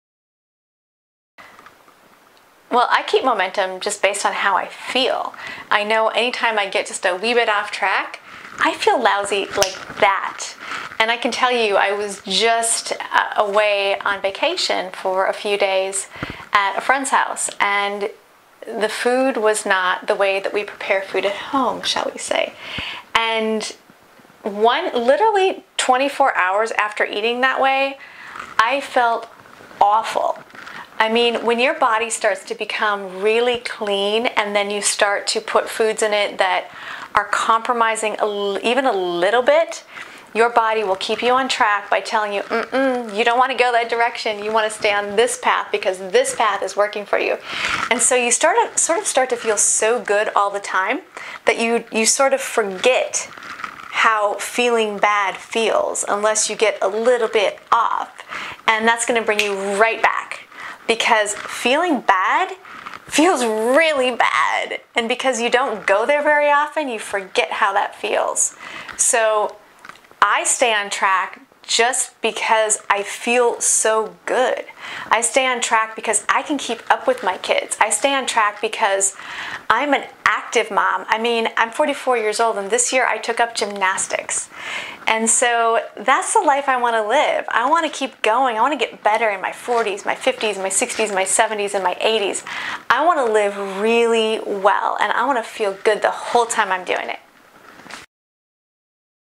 How I stay on track.
Lisa shares how stays on the right track with her diet and lifestyle.